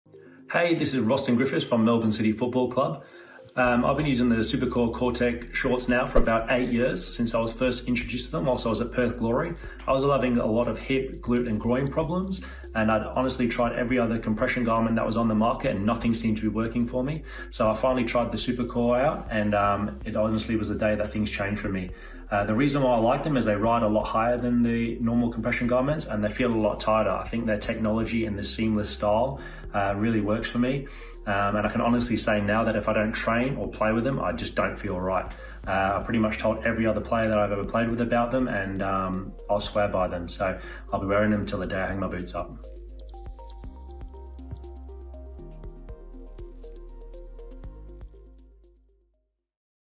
ROSTYN GRIFFITHS - Professional Soccer Player recovers from groin related injury
ROSTYN Exprience with Supacore